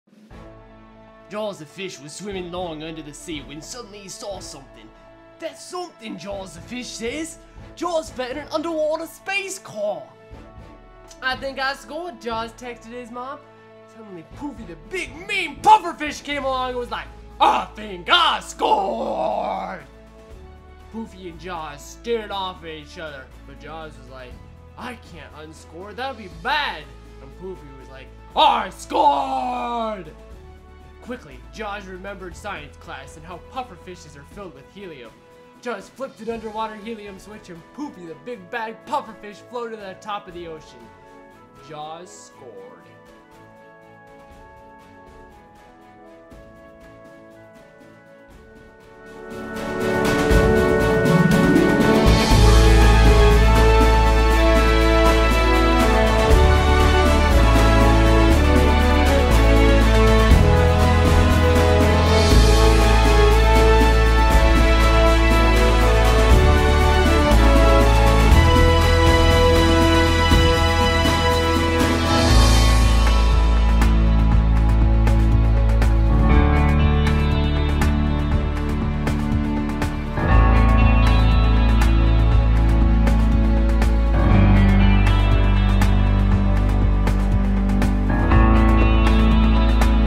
Jaws the Fish Scores!!!
Jaws the fish scores, but he runs into a big bad puffer fish...